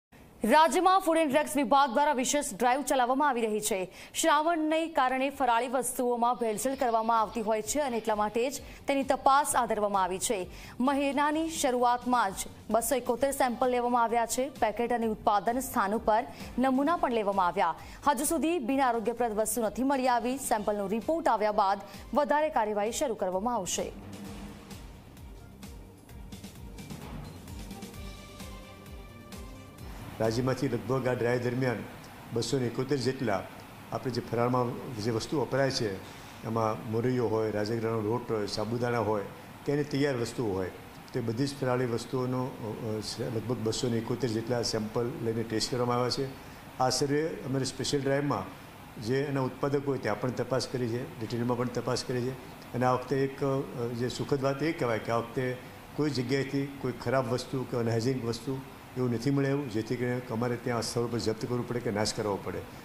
Food and Drugs Department conducts special drive during holy Shravan month in Gandhinagar
રાજ્યમાં ફૂડ એન્ડ ડ્રગ્સ વિભાગની વિશેષ ડ્રાઈવ --
શ્રાવણને ફરાળી વસ્તુમાં ભેળસેળ રોકવા વિશેષ ડ્રાઈવ --
મહિનાની શરૂઆતમાં જ 271 સેમ્પલ લેવાયા --
પેકેટ અને ઉત્પાદન સ્થાનો પરથી નમુના લેવાયા --
હજુ સુધી બિન આરોગ્યપ્રદ વસ્તુ નથી મળી --
સેમ્પલનો રિપોર્ટ આવ્યા બાદ વધુ કાર્યવાહી કરાશે --